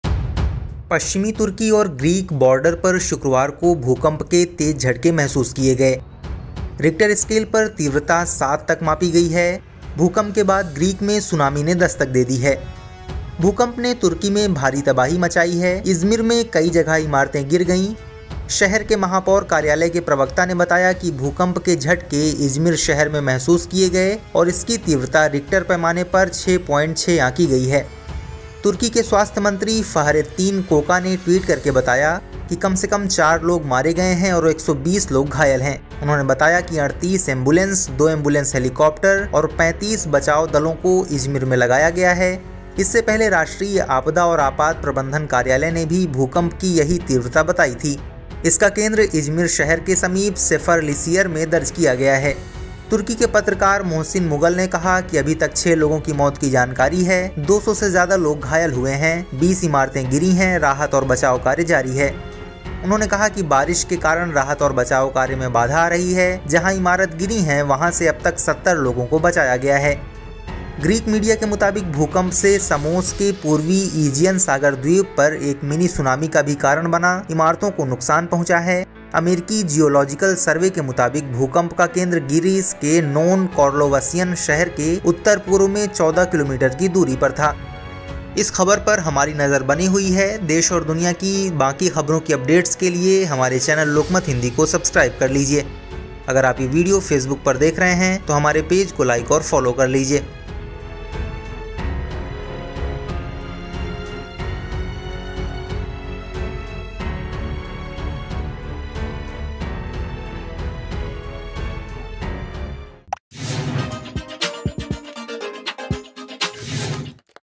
Earthquake hit Greece and Turkey: तुर्की में जोरदार भूकंप, इमारतें जमींदोज, कई लोगों की मौत
पश्चिमी तुर्की और ग्रीस बॉर्डर पर शुक्रवार को भूकंप के तेज झटके महसूस किए गए हैं। रिक्टर स्केल पर तीव्रता 7.0 तक मापी गई है. भूकंप के बाद ग्रीक में सुनामी ने दस्तक दी है. भूकंप ने तुर्की में भारी तबाही मचाई है. इजमिर में कई जगह इमारतें गिर गई हैं. शहर के महापौर कायार्लय के प्रवक्ता ने बताया कि भूकंप के झटके इजमिर शहर में महसूस किए गए और इसकी तीव्रता रिक्टर पैमाने पर  6.6 आंकी गई है।